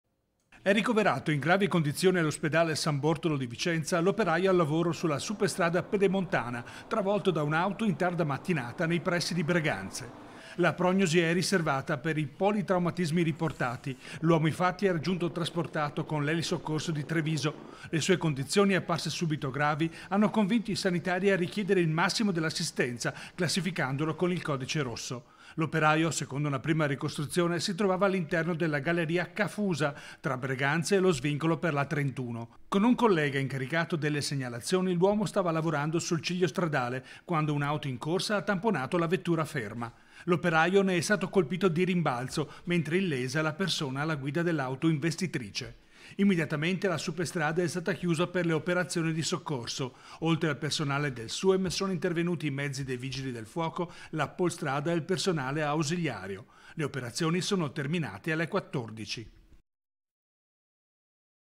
INCIDENTE SUL LAVORO LUNGO LA 'PEDEMONTANA', GRAVE UN OPERAIO | 18/10/2021
A3 NEWS Veneto 18/10/2021 - BREGANZE - Un grave incidente sul lavoro, avvenuto a metà giornata, ha bloccato il traffico sulla superstrada Pedemontana.   || E’ ricoverato in gravi condizioni all’ospedale san Bortolo di Vicenza l’operaio al lavoro sulla superstrada Pedemontana travolto da un’auto in tarda mattinata nei pressi di Breganze. La prognosi è riservata per politraumatismi riportati, l’uomo infatti è giunto trasportato con l’elisoccorso di Treviso. Le sue condizioni apparse subito gravi, hanno convinto i sanitari a richiedere il massimo dell’assistenza classificandolo con il codice rosso. L’operaio, secondo una prima ricostruzione, si trovava all’interno della galleria Ca’ Fusa tra Breganze e lo svincolo per l’A31. Con un collega incaricato delle segnalazioni l’uomo stava lavorava sul ciglio stradale quando un una auto in corsa ha tamponato la vettura ferma.  L’operaio ne è stato colpito di rimbalzo mentre illesa è la persona alla guida dell’auto investitrice. Immediatamente la superstrada è stata chiusa per le operazioni di soccorso. Oltre al personale del Suem sono intervenuti i mezzi dei Vigili del Fuoco , la Polstrada e il personale ausiliario. Le operazioni sono terminate alle 14.00. (Servizio di Daniele Garbin) - Segui Antenna Tre anche sul digitale terrestre! Visita il sito